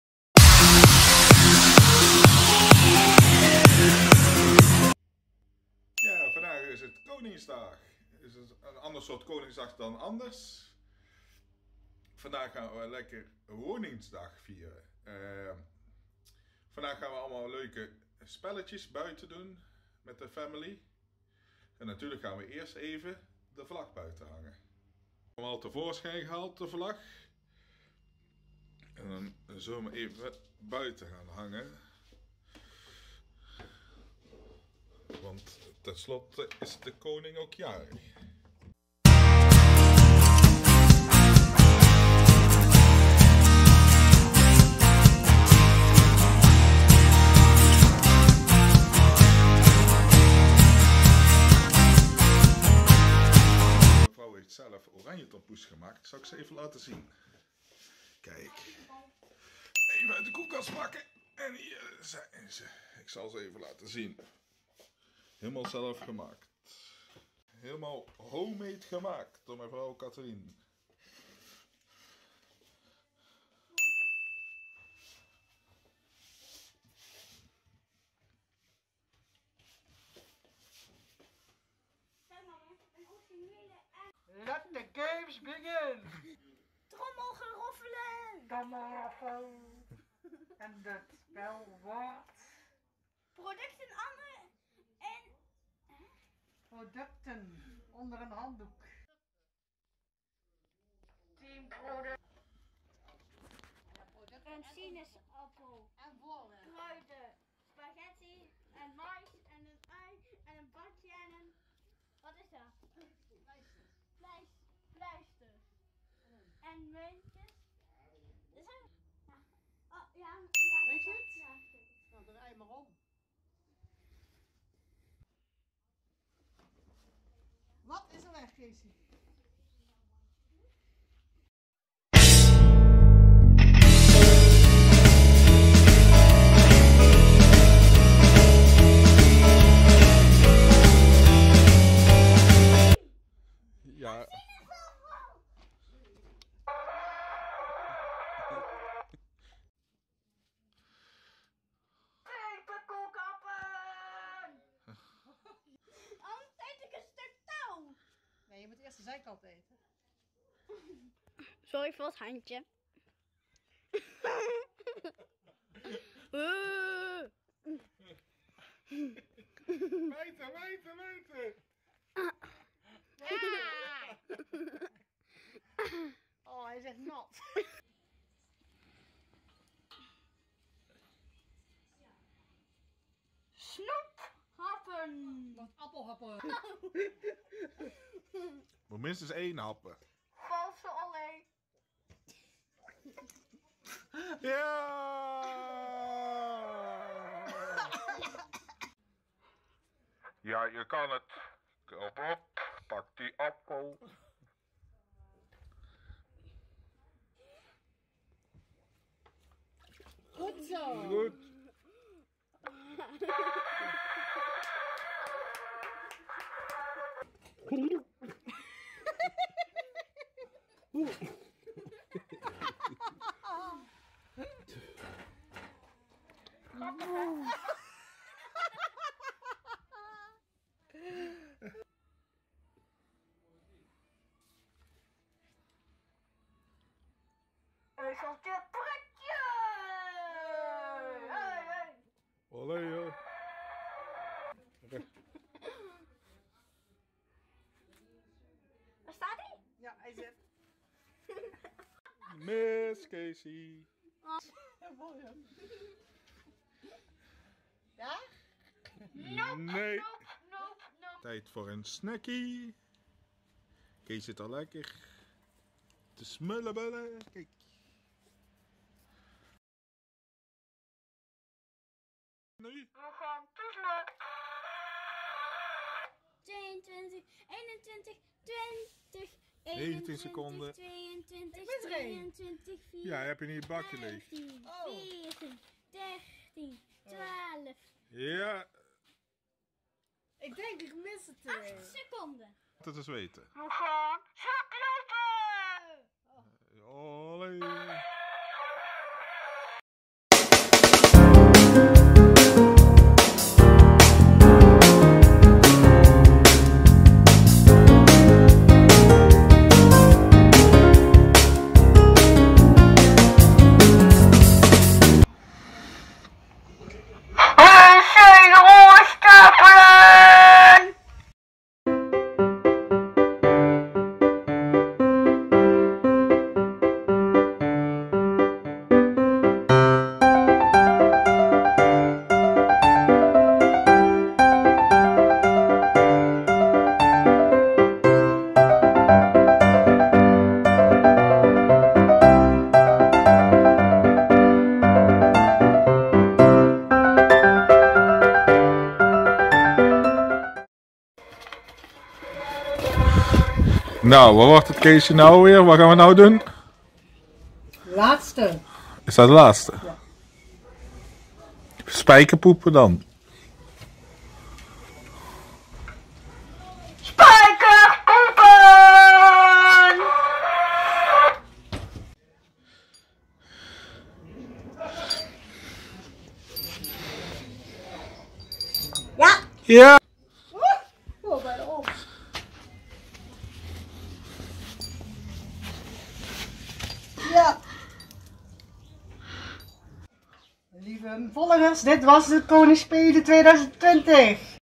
(w)konings spelen in de achtertuin carona proof //run_with_robbert
Dit ja jaar de koning spelen in de achtertuin gespeeld.
Het was prachtig weer en we moesten toch thuis blijven door de carona.
Hadden we een heel programma in elkaar gezet met oud Hollandse spelletjes.
Zoals spijkerpoepen,koek happen, zak lopen, ezeltje prikje.

Instagram.com/run_with_robbert_slagers